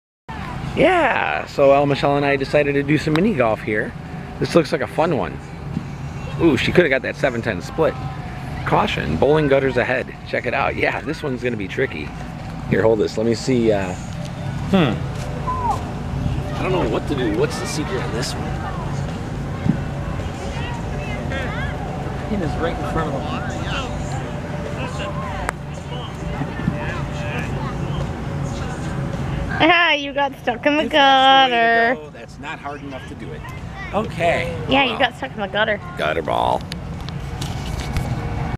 Mini Golf Bowling Lane Hole Badger Sports Park Appleton, WI 9-9-17
This is one of the more unique holes at this mini golf course. Obviously L. Michelle and I don't know the secret to this hole!